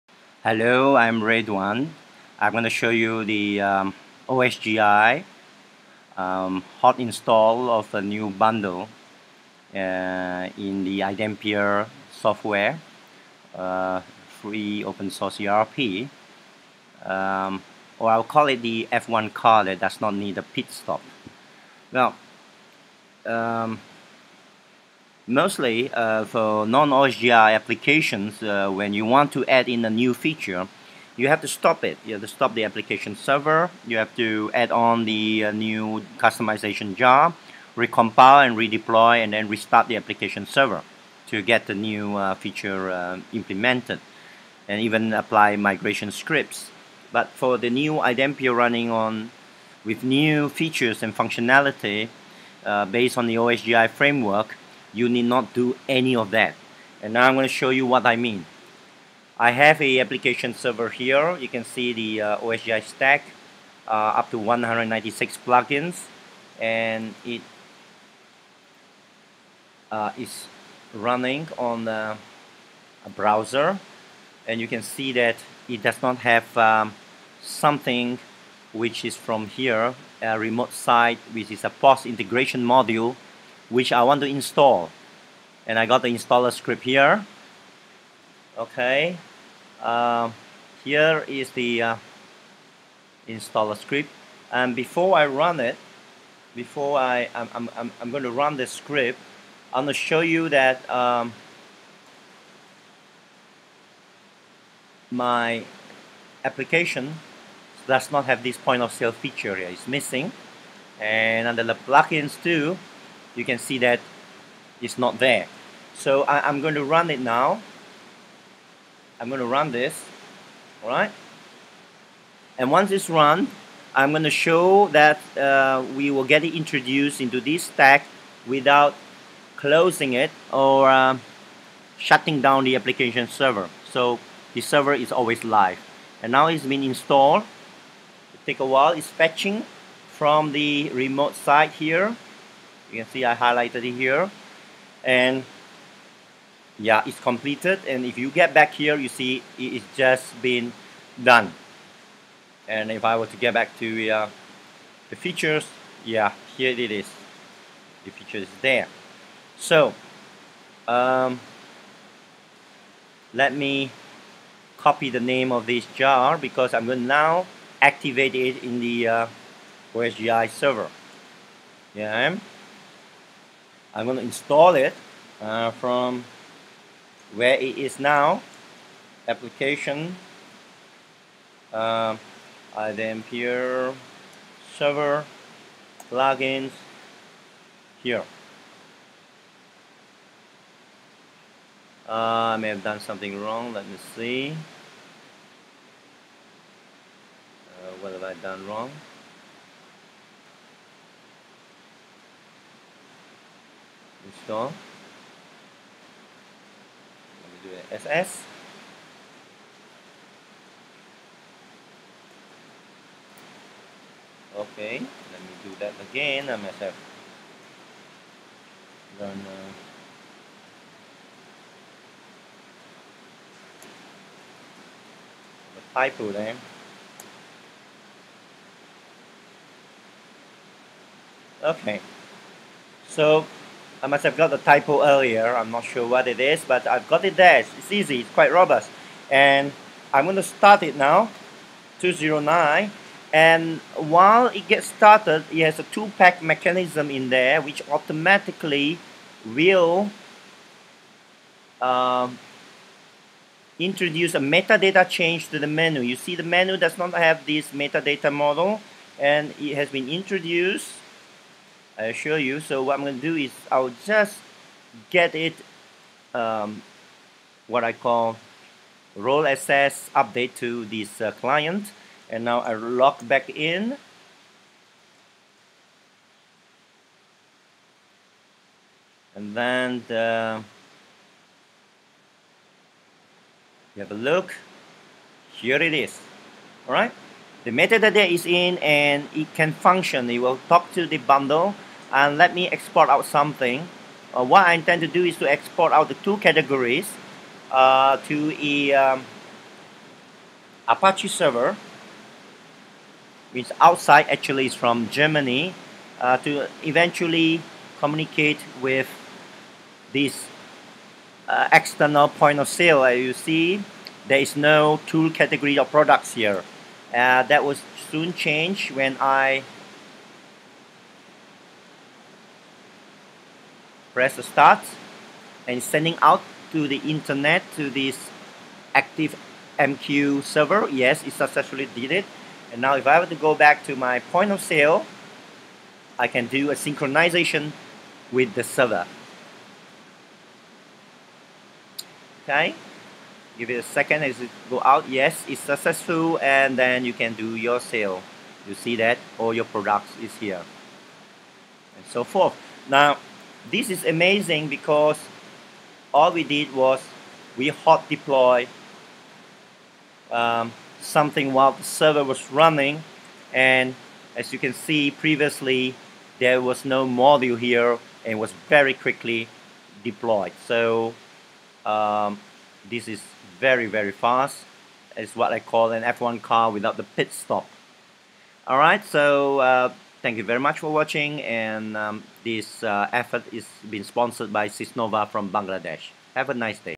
Hot Deploy New OSGi Bundle
OSGi Hot Install of New Bundle in iDempiere or The F1 Car That Does Not Need A Pit-stop!
Imagine you are in the iDempiere F1 car while your competitors need to do a pit stop for adding a new feature, open up the source-code, place difficult to identify and clumsy and jumbled up customization.jar then recompile, and download and apply migration scripts, and redeploy and restart to see the new feature running, but you just whiz past all that without a pit stop!  Yes, you can add-on a feature with just 2 lines via OSGi, the magic framework from IBM Equinox.

The extra two lines to type in the OSGi console are
 install (location and name of bundle)
 start (bundle id)

Of course you can choose to close the OSGi apps server and run again for the bundle to be installed on its own without those two lines.